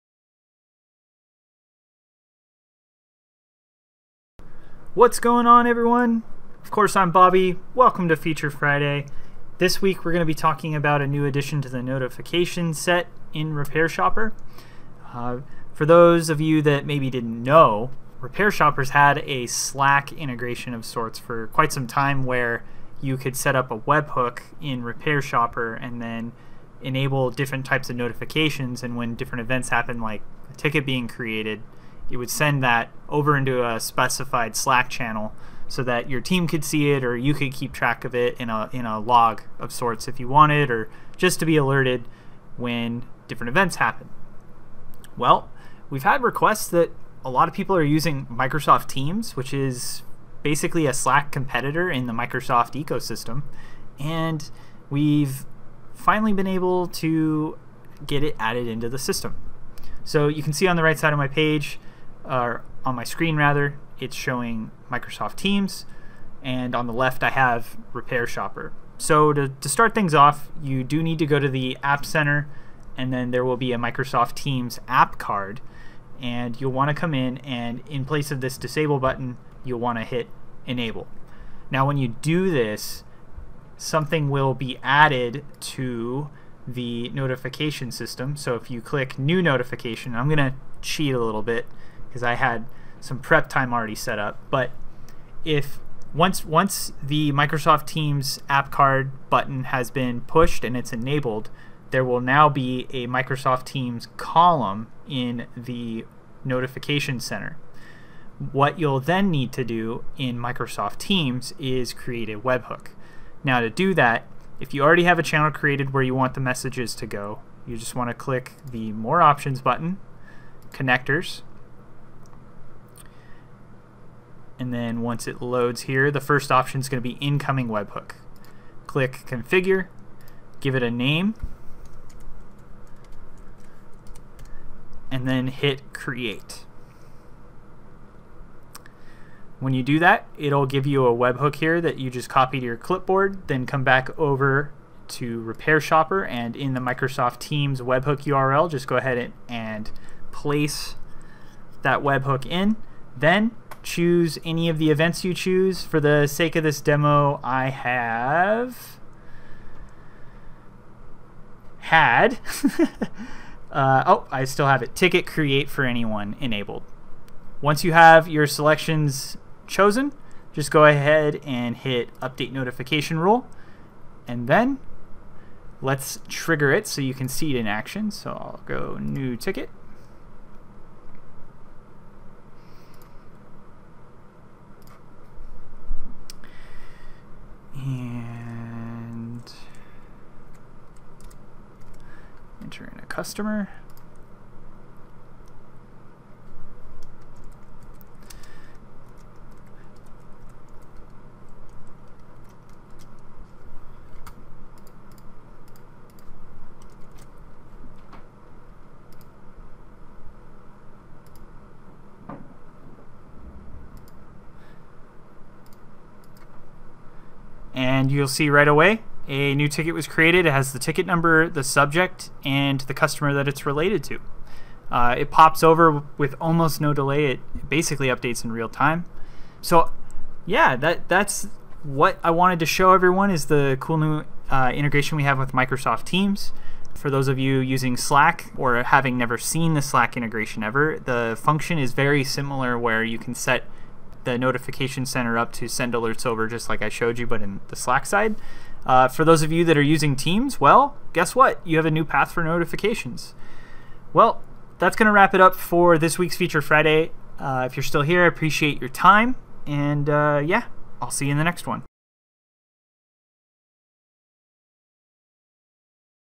Feature Friday - Microsoft Teams Integration Added!
In this video, Bobby walks through the newly added Microsoft Teams integration which allows you to set up notifications that trigger message creation in your Microsoft Teams account. Watch on to learn more!